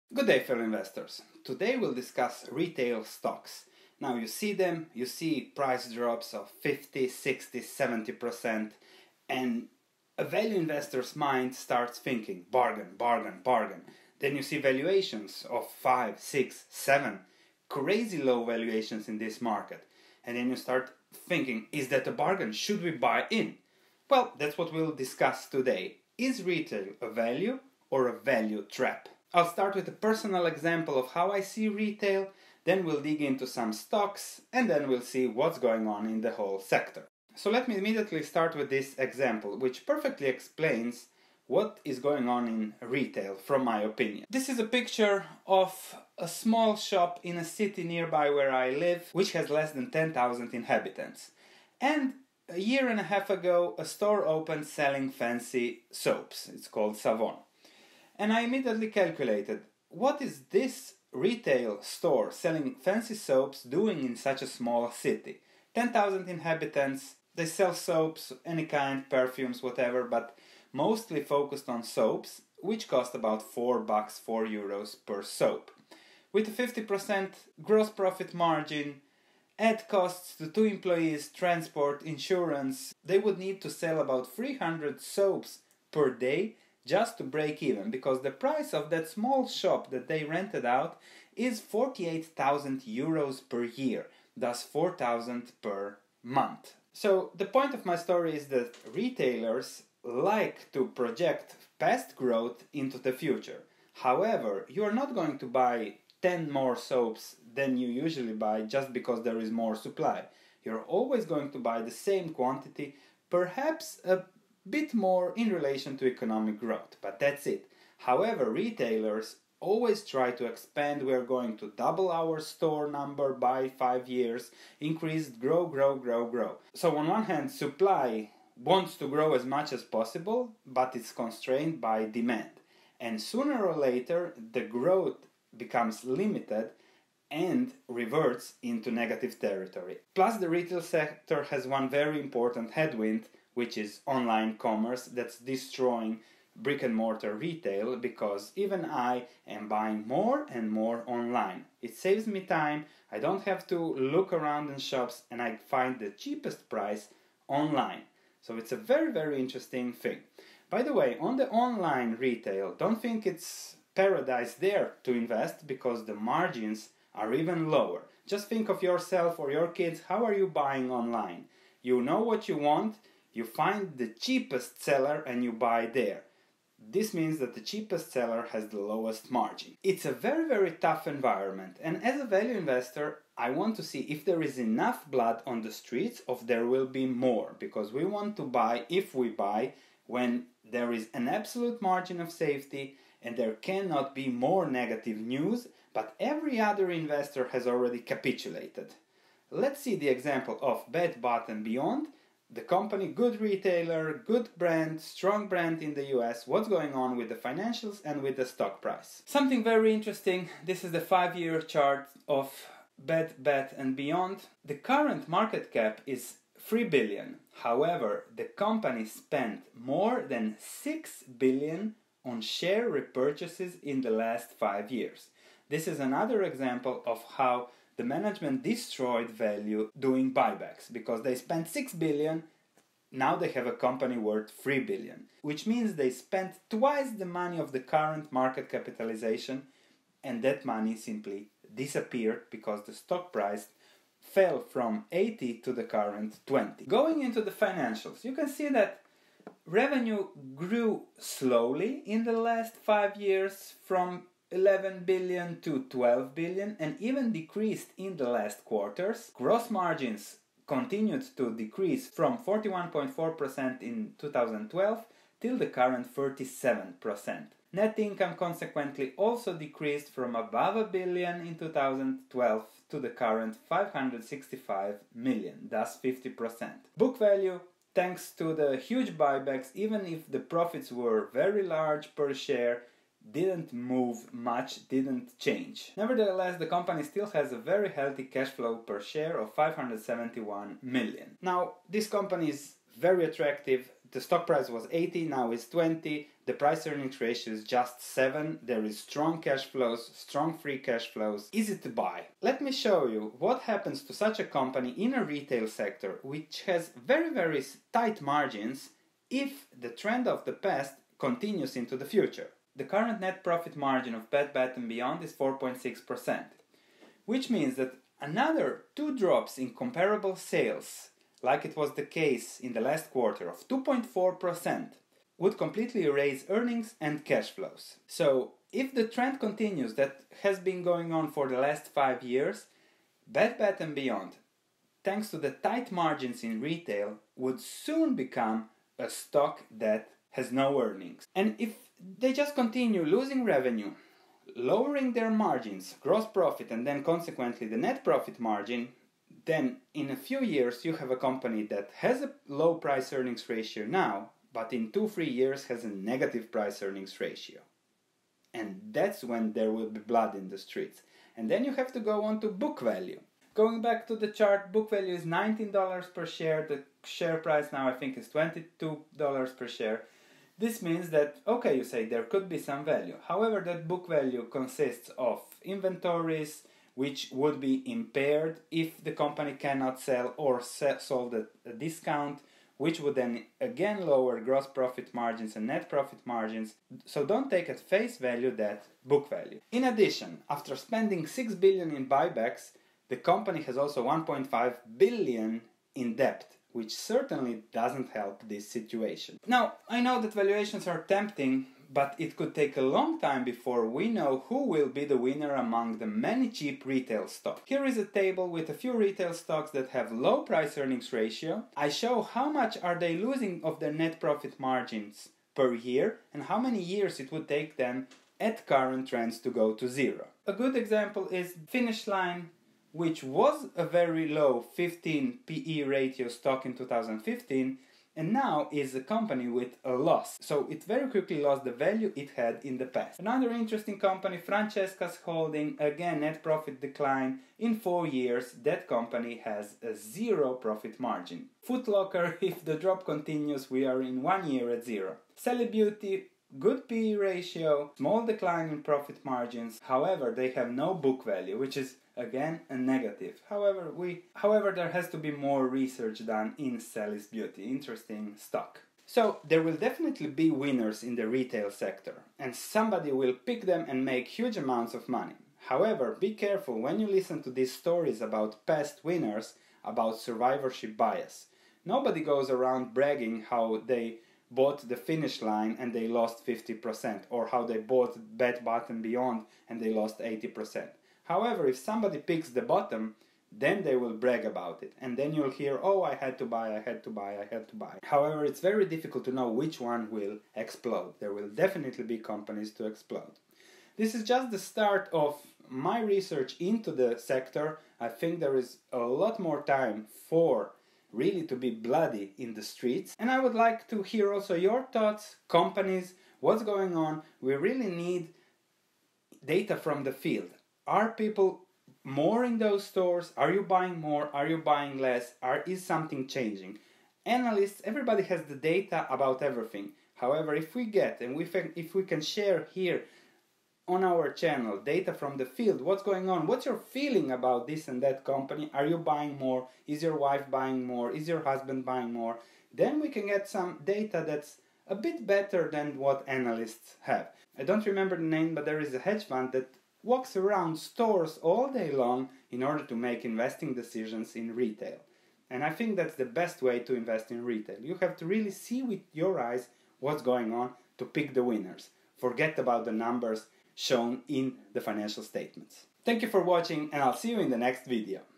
RETAIL STOCKS ARE CHEAP - VALUE OR VALUE TRAP?
I am also a book author:
Modern Value Investing book:

Retail stocks are getting cheaper day by day. I discuss my view of the retail environment, stocks like Bed Bath And Beyond (NYSE: BBBY), discuss profit margins and book values in order to see if there is more downside or that is it. It is possible to make money we have to keep our eyes open.
There is also a very important headwind, online commerce.